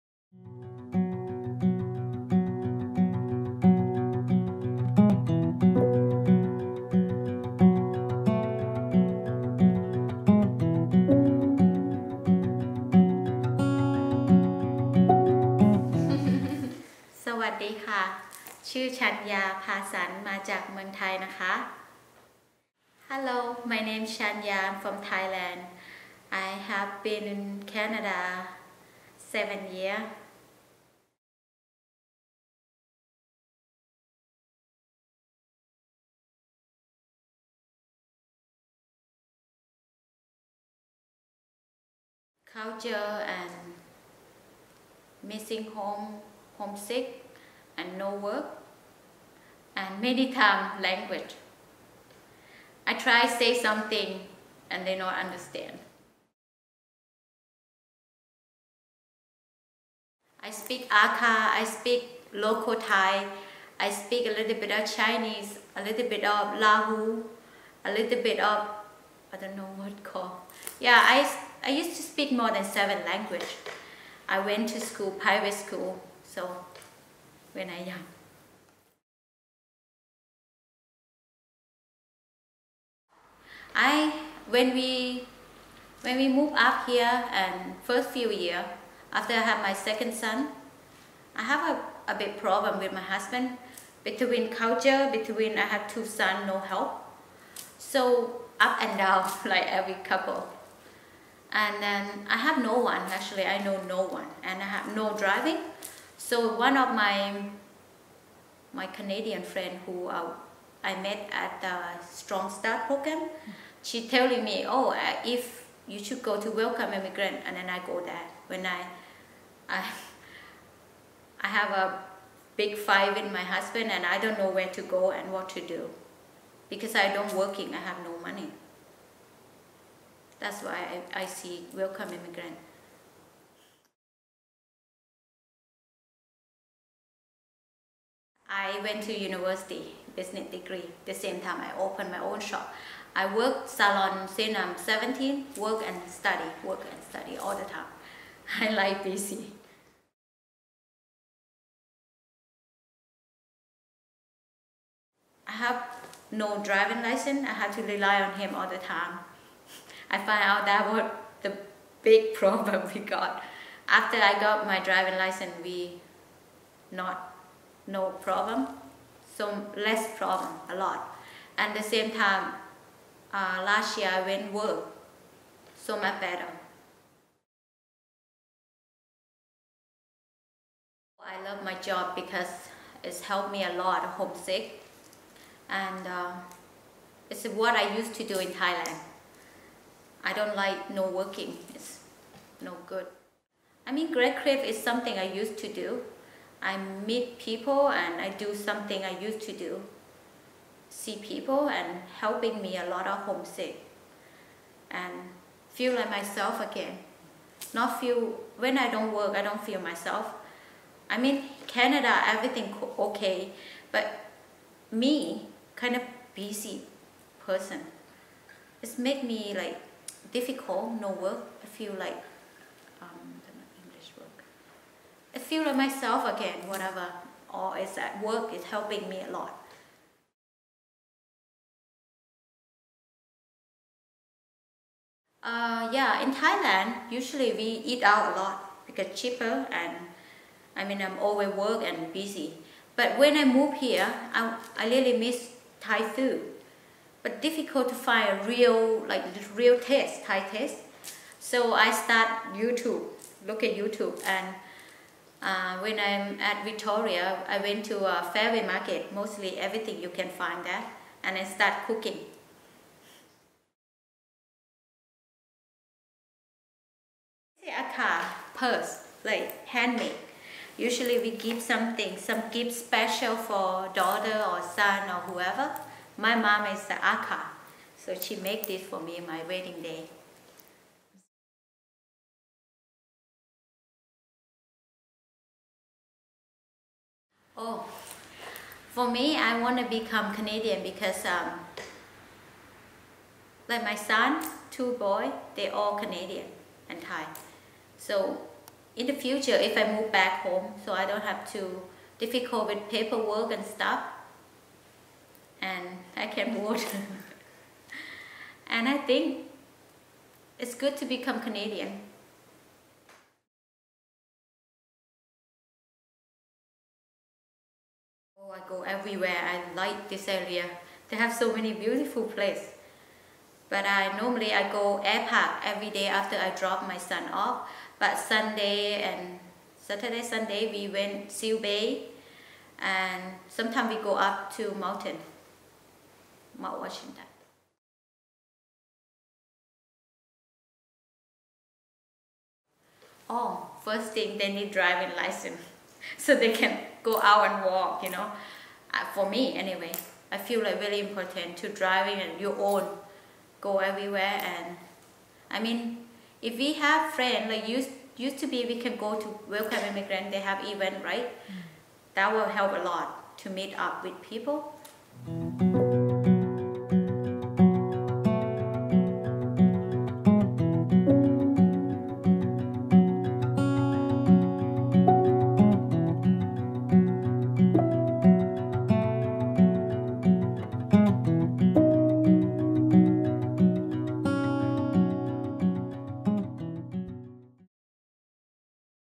TheresMoreToMe - Chanya
A Welcoming Communities Coalition (Immigrant Welcome Centre – North Island) and CV/Arts partnership.

This is video one of three for TheresMoreToMe, a series that features the immigration stories generously shared by newcomers to the Valley. We are celebrating diversity in the Comox Valley and hope that these stories help newcomers in our community feel less alone.